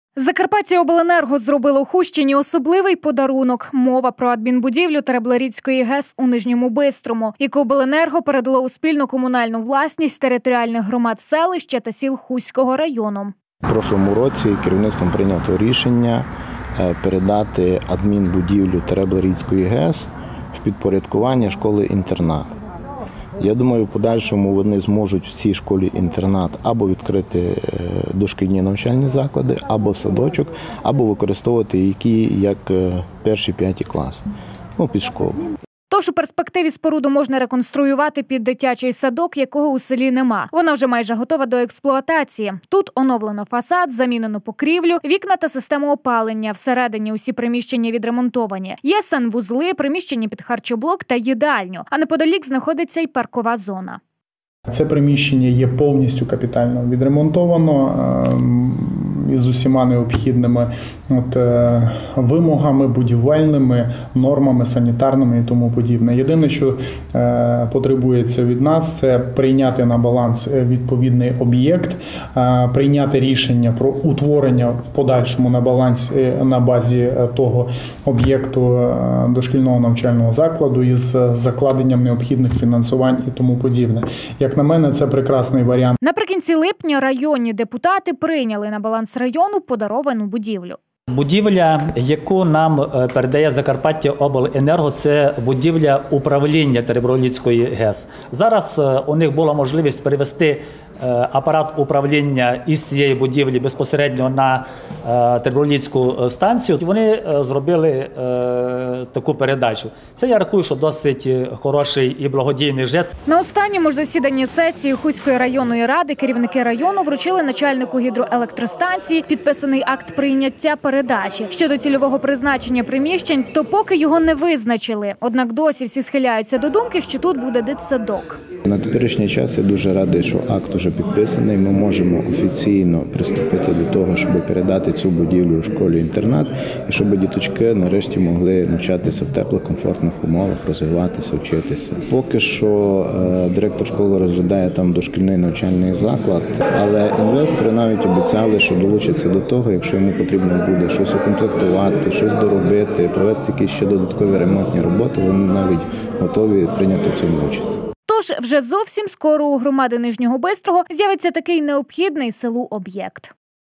Будівля в подарунок
: «Закарпаттяобленерго» зробило Хустщині особливий подарунок. Мова про адмінбудівлю Теребле-Ріцької ГЕС у Нижньому Бистрому, яку Обленерго передало у спільну комунальну власність територіальних громад селища та сіл Хустського району. Щодо цільового призначення приміщень, то поки його обранці не визначили. Ймовірно, там розмістять дошкільний навчальний заклад, якого у селі нема.